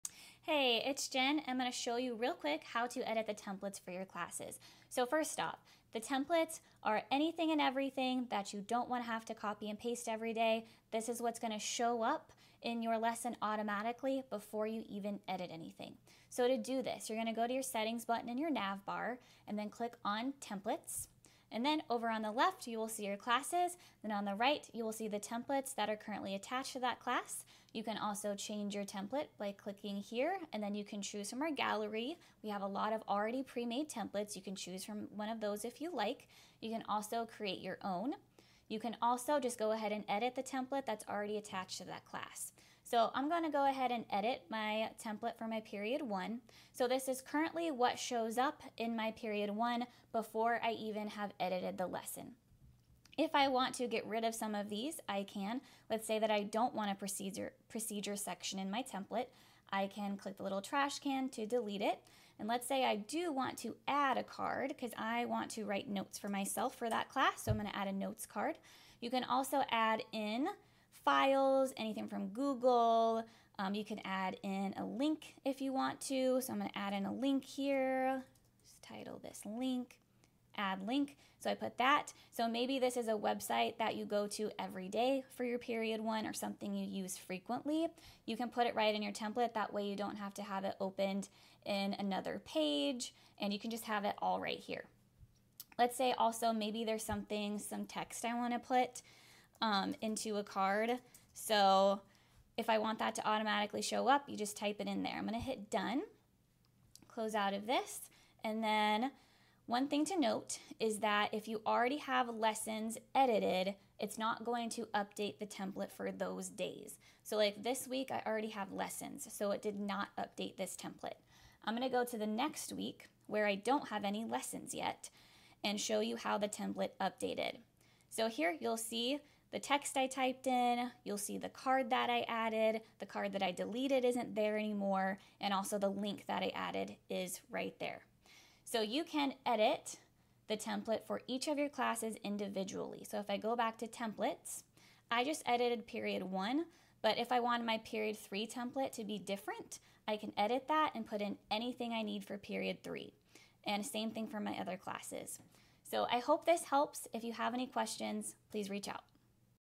Setting Up Time-Saving Lesson Plan Templates in Common Planner: Step-by-Step Teacher Hack
Templates save time and create consistency. Learn how to create your own or use prebuilt ones to streamline your planning process.